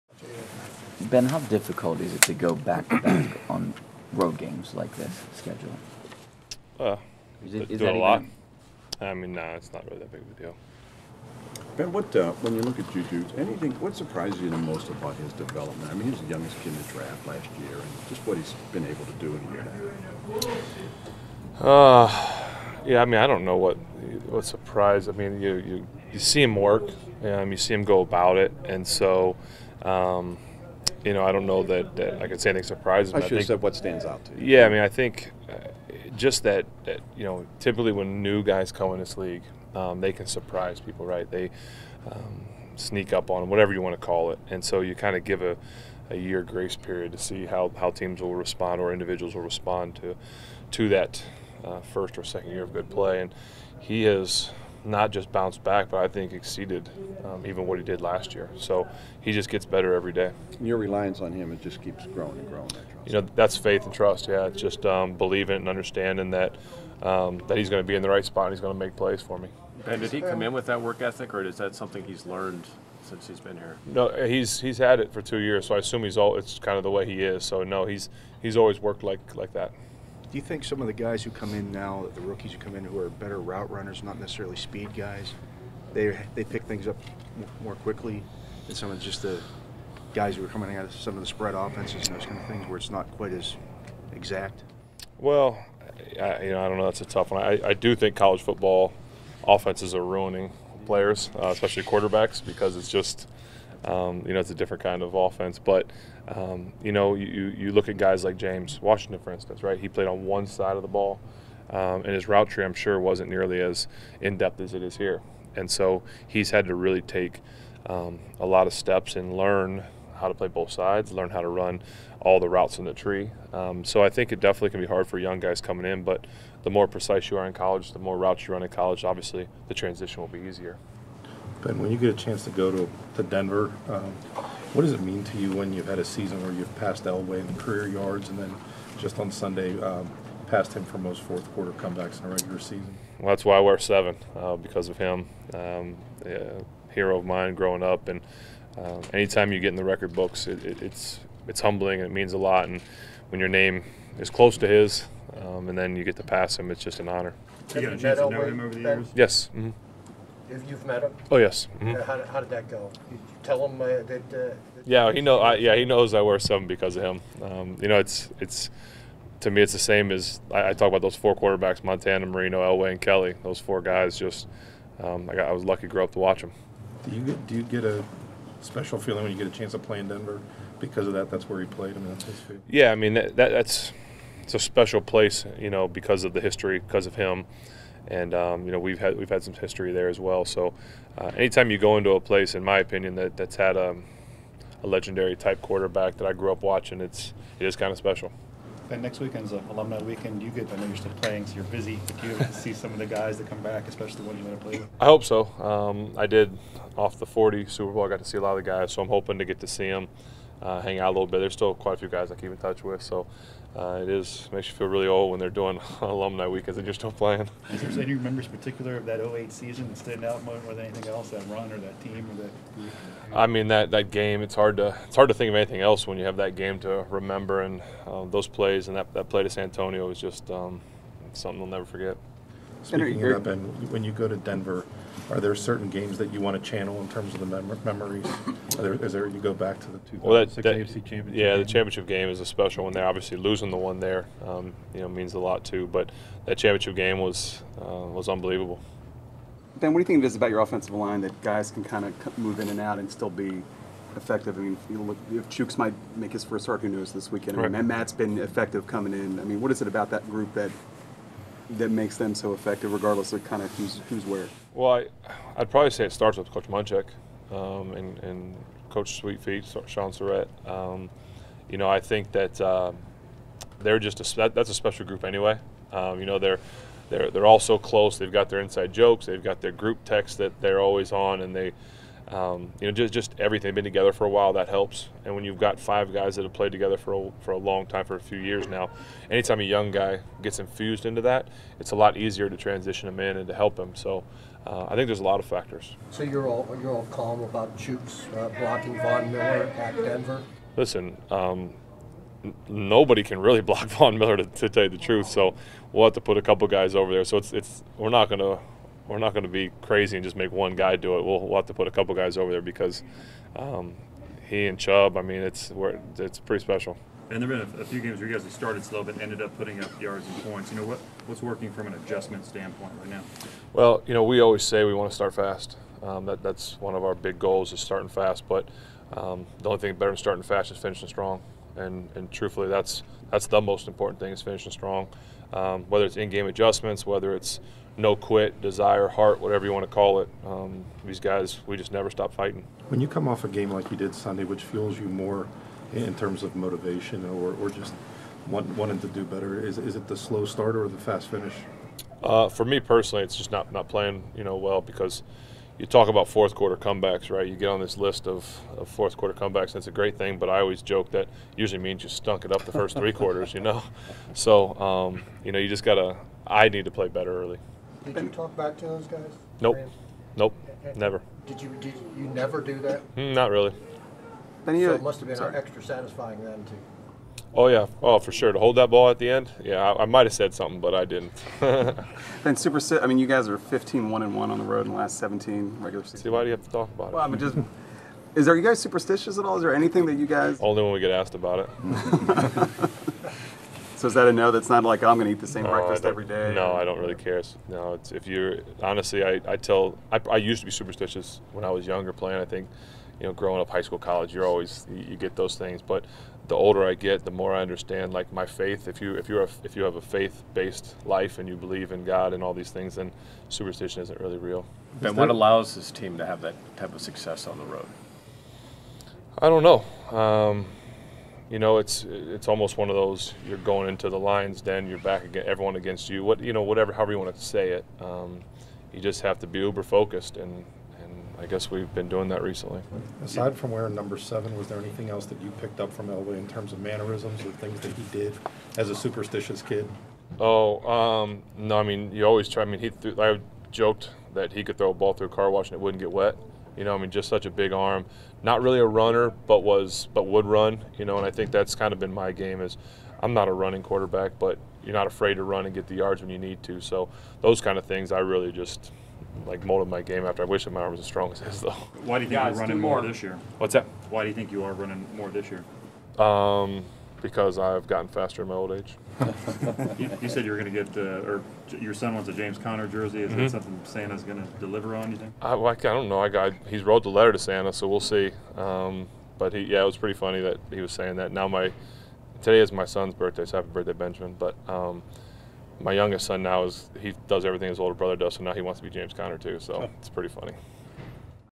Ben Roethlisberger on John Elway, Broncos | Pittsburgh Steelers
Pittsburgh Steelers QB Ben Roethlisberger discusses John Elway, the Broncos defense and the depth on the offensive line.